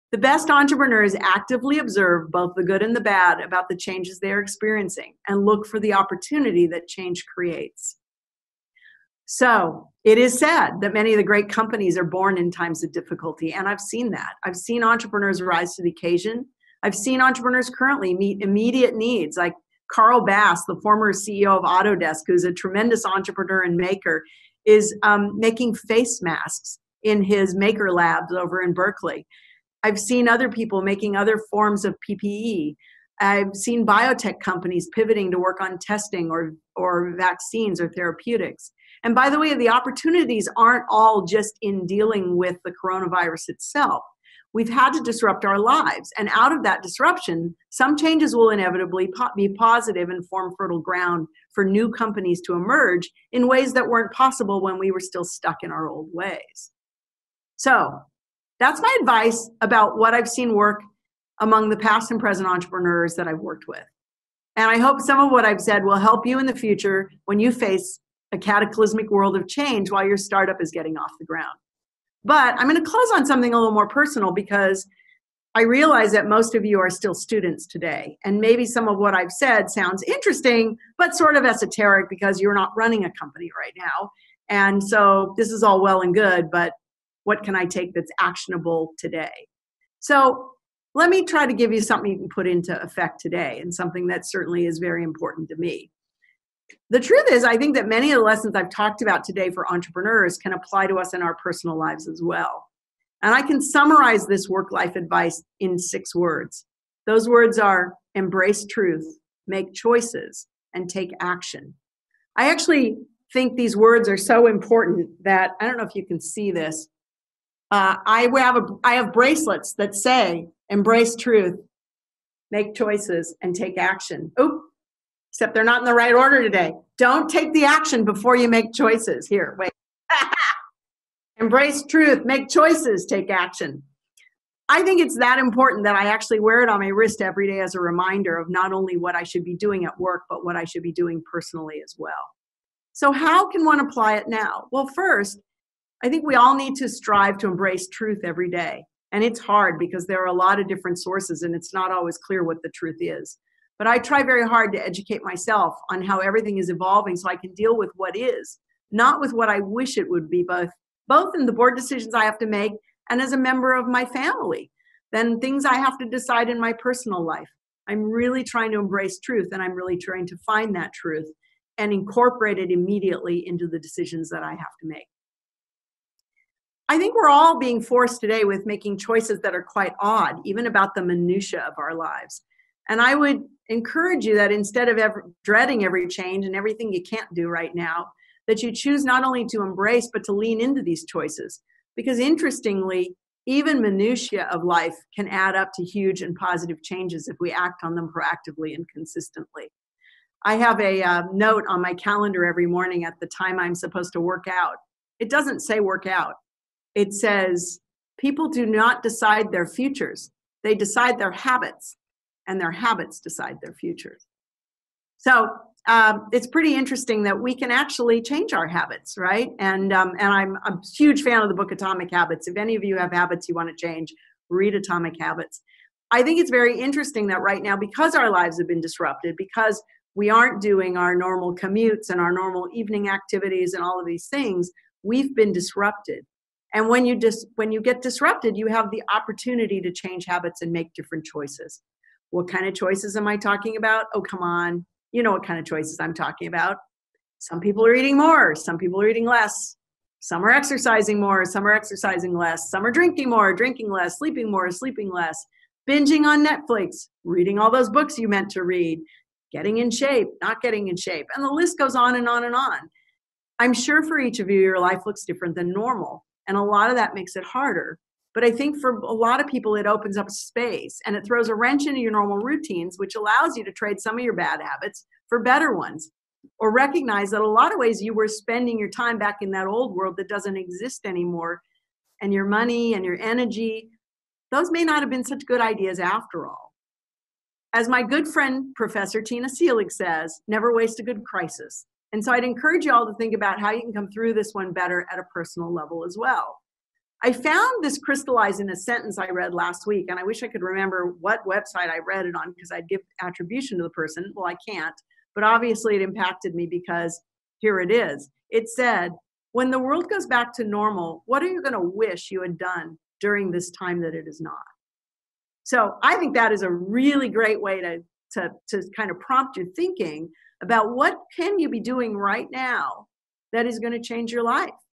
Finding Opportunity in Change
Skilled entrepreneurs seem to have a unique ability to find opportunity even amid drastic change, observes Threshold Ventures partner Heidi Roizen. This mindset isn’t just for entrepreneurs launching startups, she adds. Each of us has the opportunity to use a crisis to change our habits, and thereby change our futures.